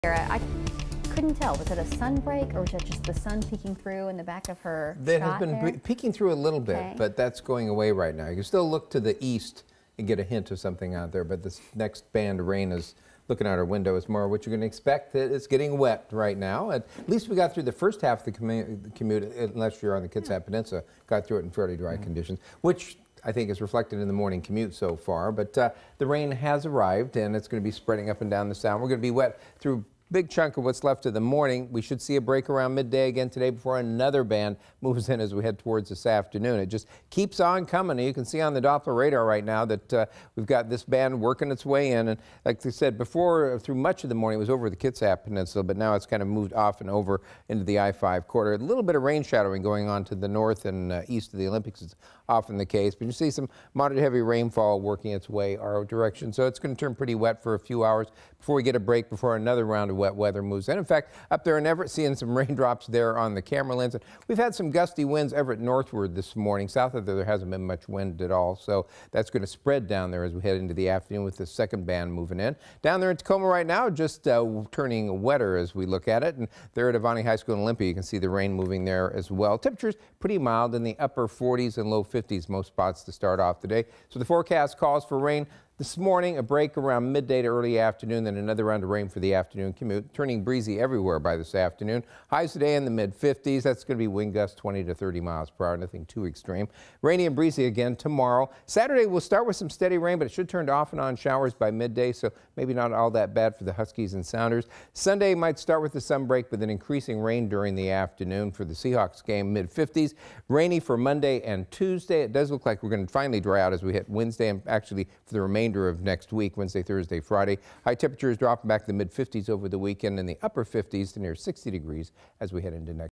KING 5 Weather
10/17/19 late morning forecast with KING 5 Meteorologist Rich Marriott